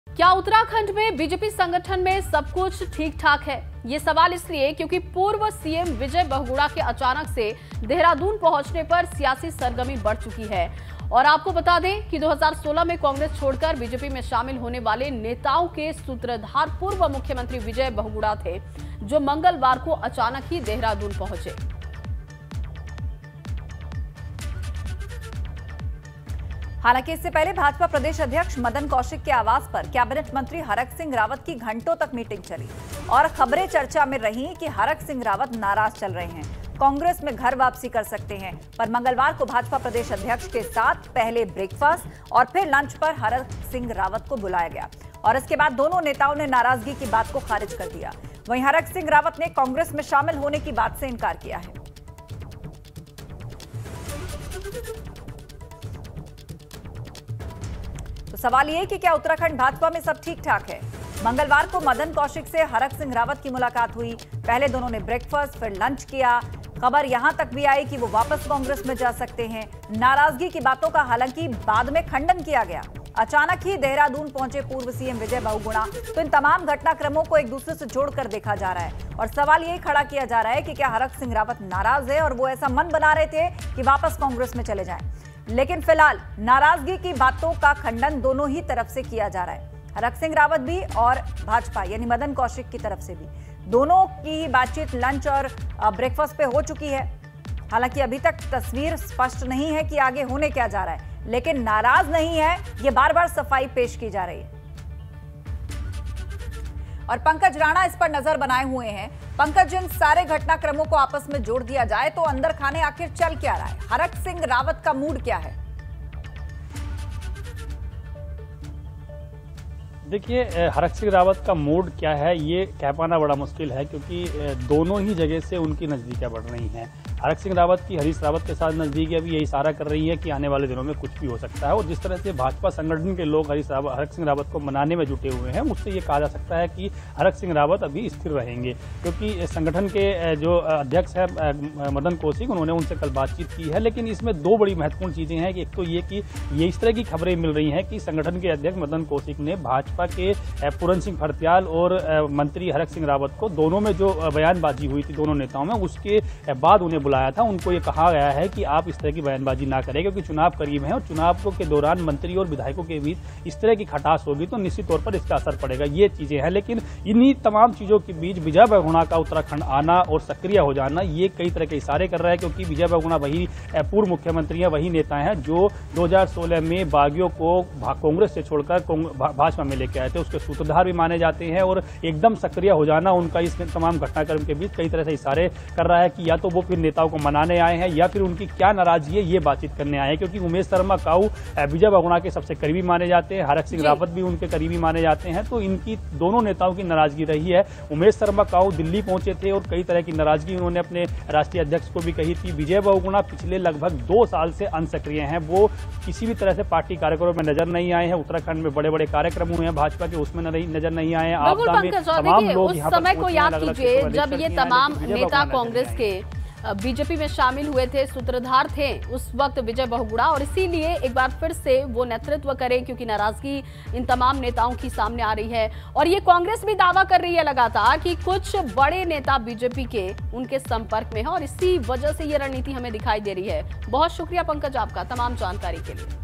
Uttarakhand: पूर्व सीएम Vijay Bahuguna अचानक पहुंचे Dehradun, क्या BJP में सब ठीकठाक है ?
क्या उत्तराखंड में बीजेपी संगठन में सब ठीकठाक है ? ये सवाल इसलिए क्योकि पूर्व सीएम विजय बहुगुणा के अचानक देहरादून पहुंचने पर सियासी सरगर्मी बढ़ चुकी है। आपको बता दें की 2016 कांग्रेस छोड़कर बीजेपी में शामिल होने वाले नेताओं के सूत्रधार पूर्व सीएम विजय बहुगुणा हैं, जो मंगलवार को अचानक ही देहरादून पहुँच गए।